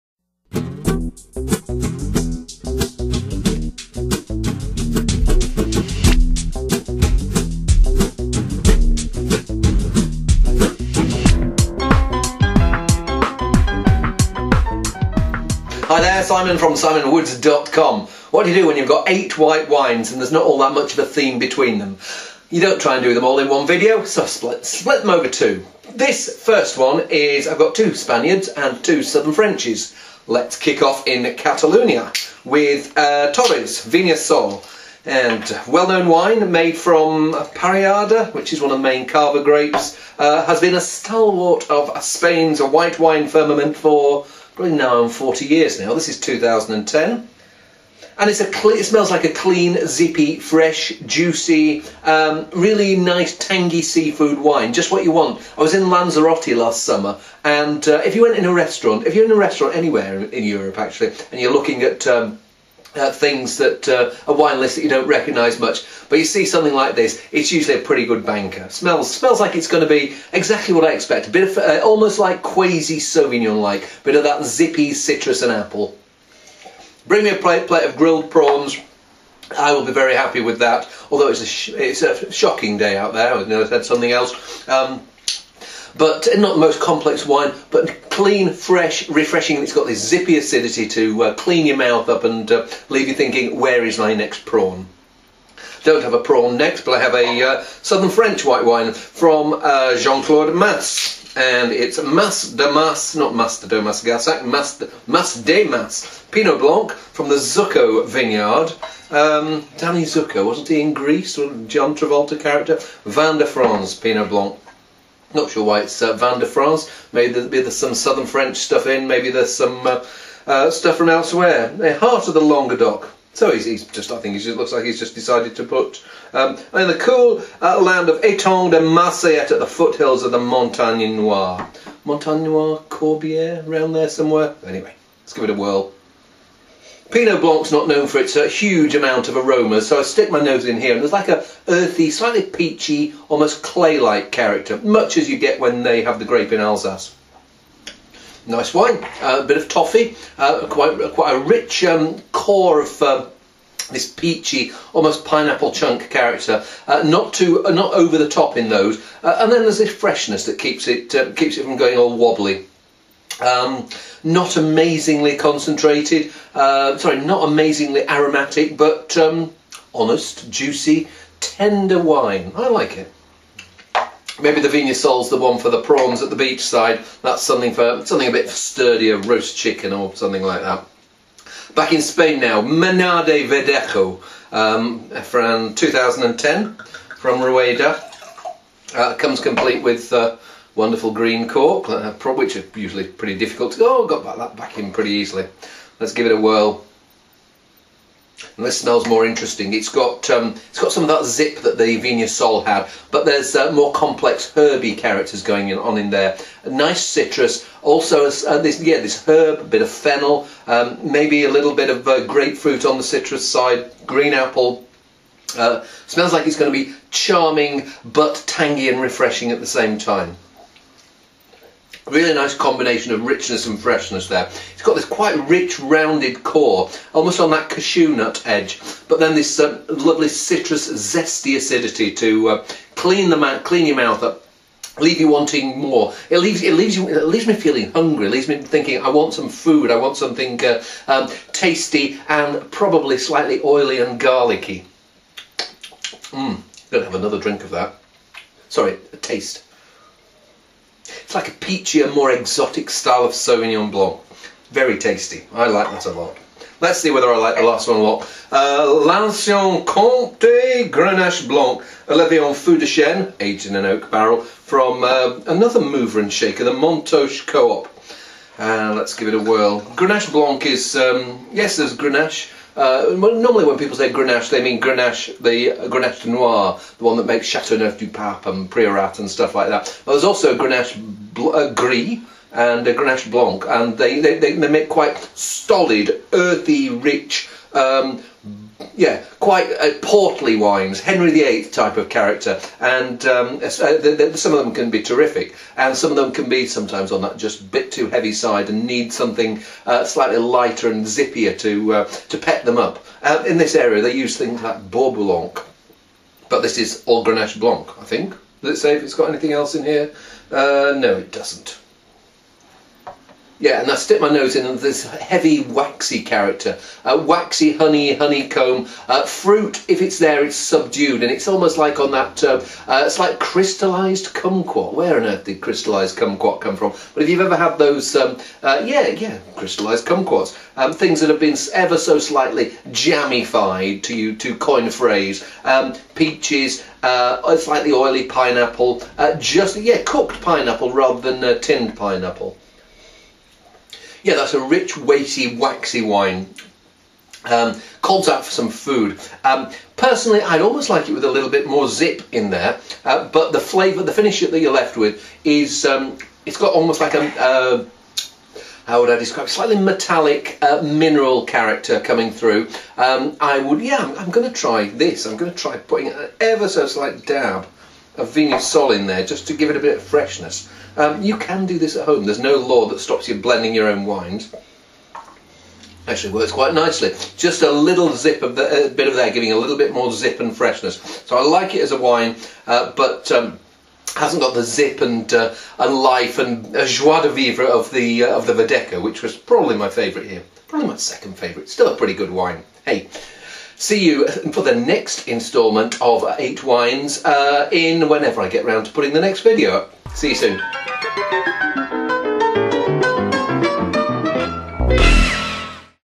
Simon Woods Wine Videos: Assorted whites from Spain & Southern France
Neither Grenache Blanc, Pinot Blanc, Verdejo nor Parellada feature on most people's list of Top Ten grape varieties. But they all feature in this video and as Simon Woods relates, they can be pretty good too...